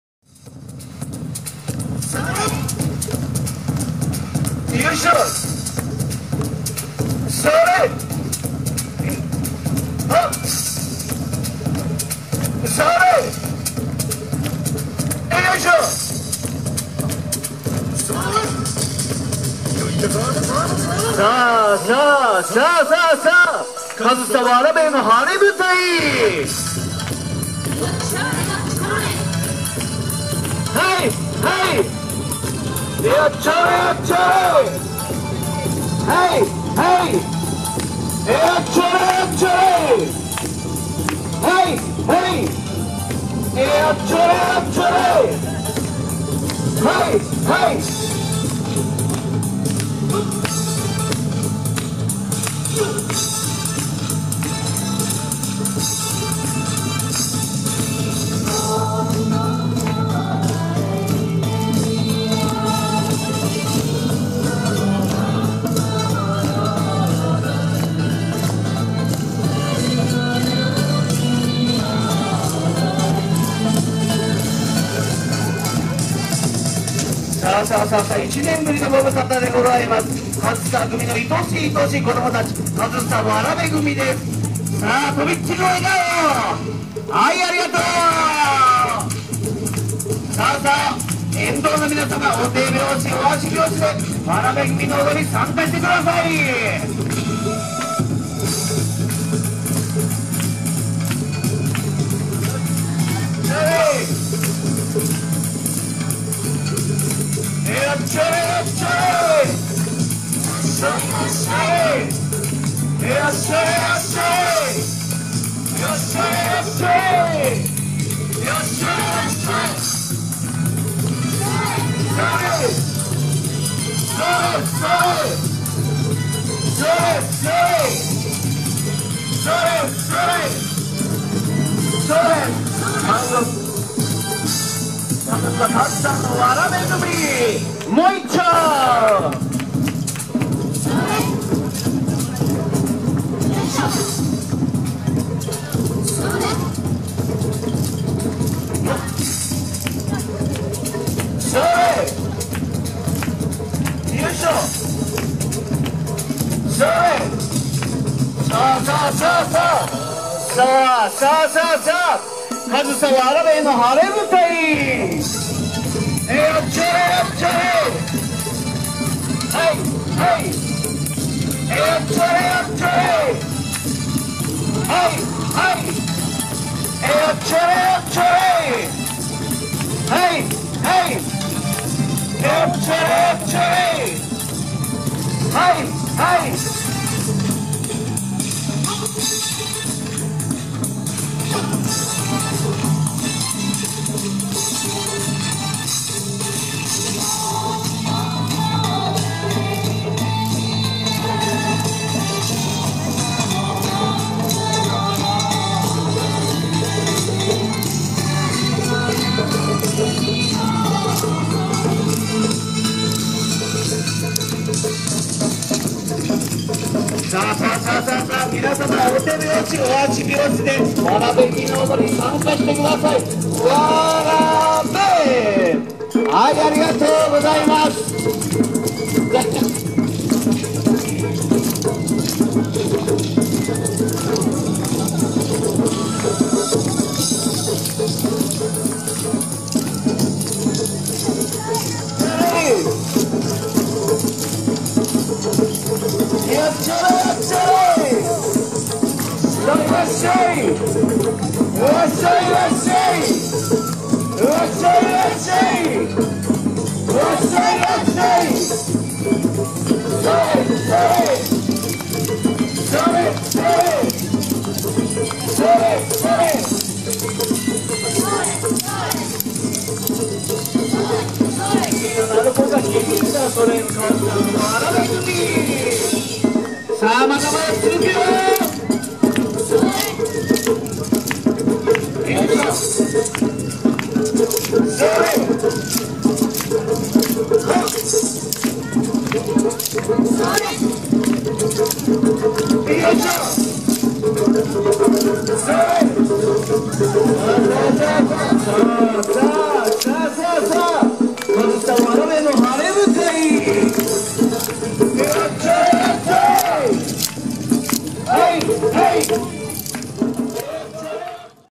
越生まつり２０１６：よさこいの部「上総童組（かずさわらべぐみ）」埼玉県入間郡越生町
映像が不鮮明なのは、見物人が多数うつりこんでいるため、

ぼかしをいれています。